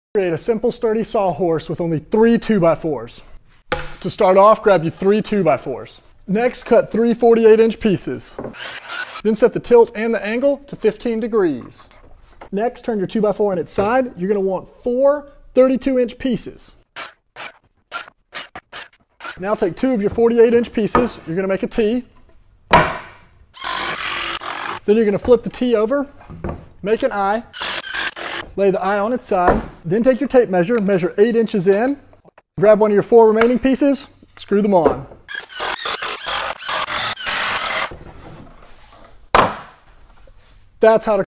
Build a saw horse that’s sturdier and larger and requires only 3 2x4s . . . Did this video after so
Build a saw horse that’s sturdier and larger and requires only 3 2x4s
Did this video after someone in the comments mentioned some edits that may be good!